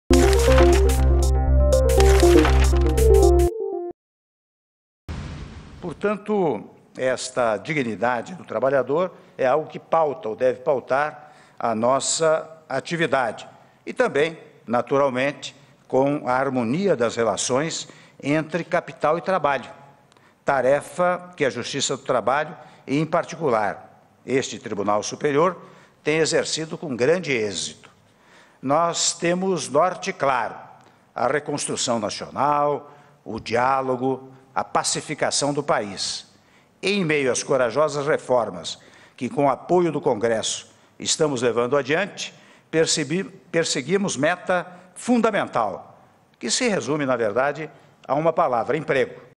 Para Michel Temer, dignidade do trabalhador deve pautar atividade do governo
REPÓRTER NBR - 24.11.16: Presidente Michel Temer diz que a dignidade do trabalhador deve pautar a atividade do governo, com a harmonia das relações entre capital e trabalho. Michel Temer participou do Seminário Comemorativo dos 75 anos da Justiça do Trabalho e 70 anos do TST em Brasília.